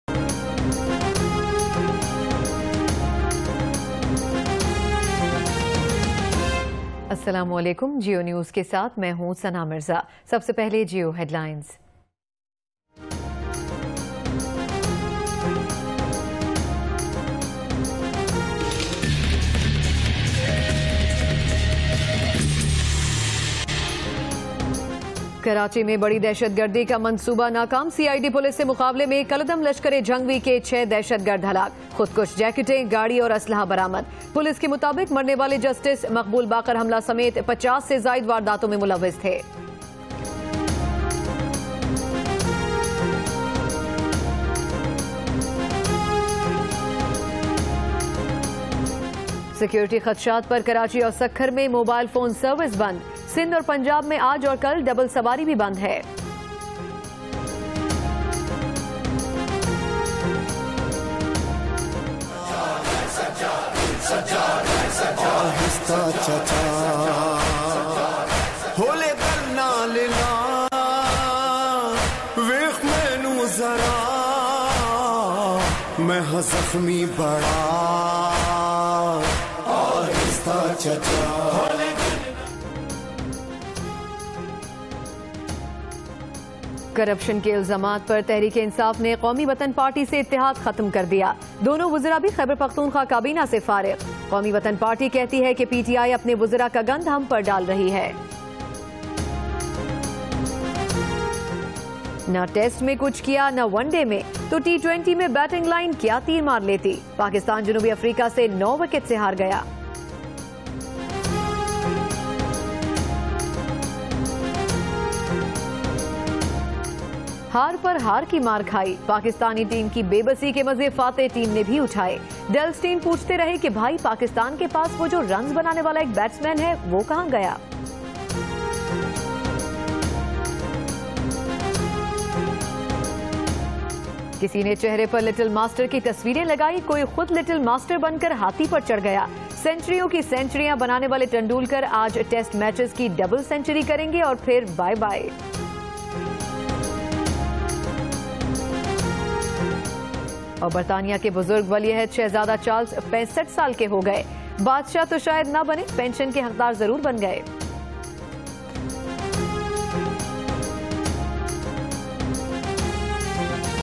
Geo Headlines-14 Nov 2013-0800
Karachi: Six members of outlawed Lashkar-e-Jhangvi killed in Mauripur